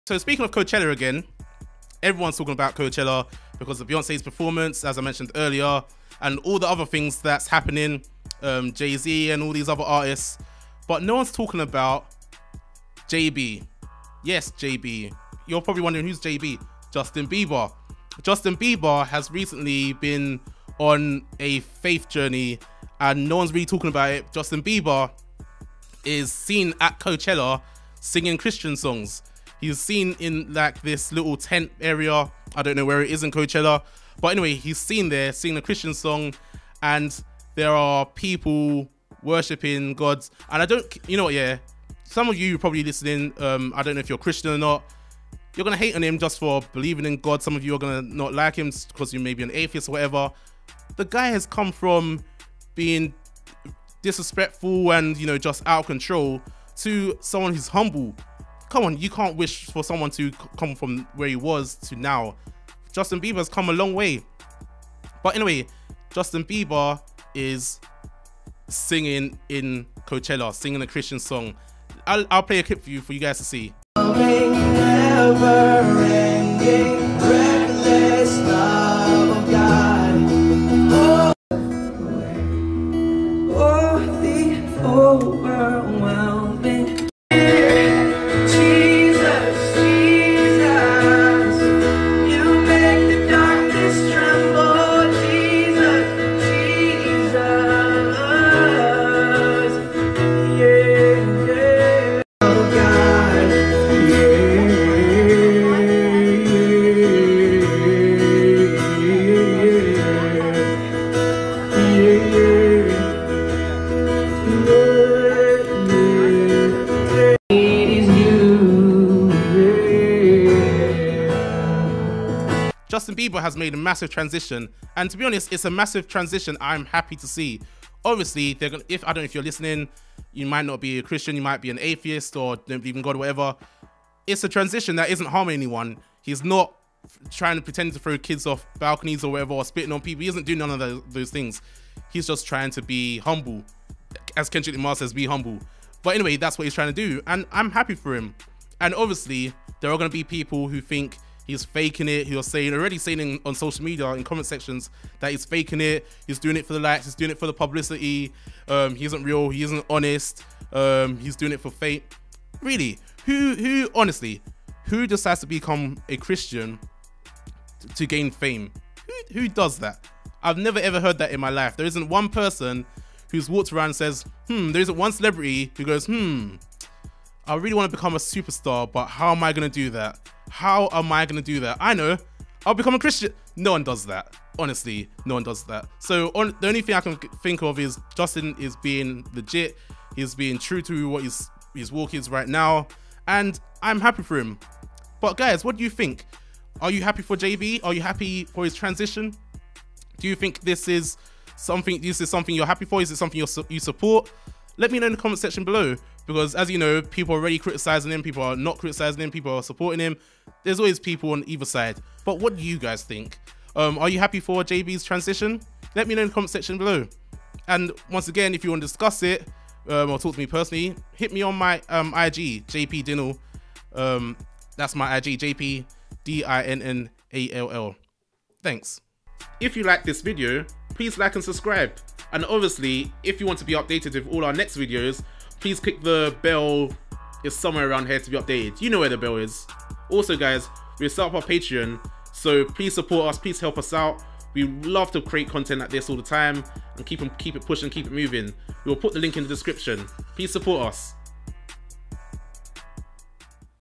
Justin Bieber Takes Coachella to Church
Justin Bieber surprises everyone at Coachella and sings a Christian song.